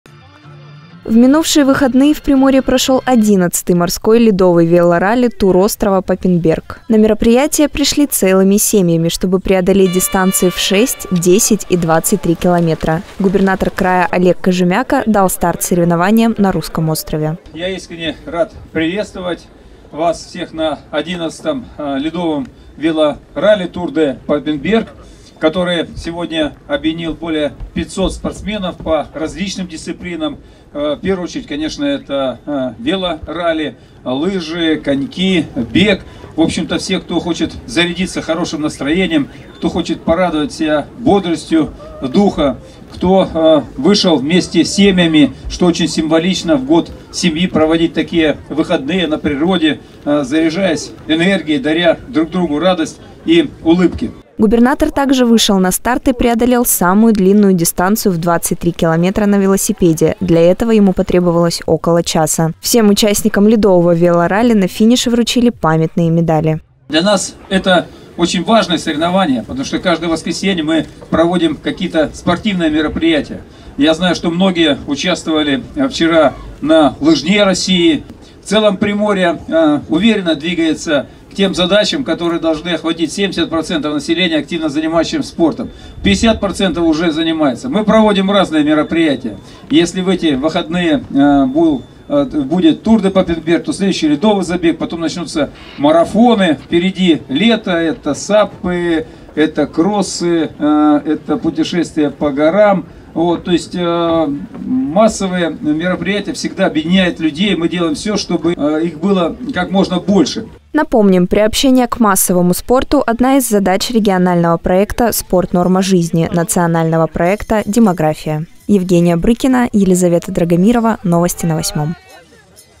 Велоралли «Тур острова Папенберг»
В минувшие выходные в Приморье прошел одиннадцатый морской ледовый велоралли «Тур острова Папенберг».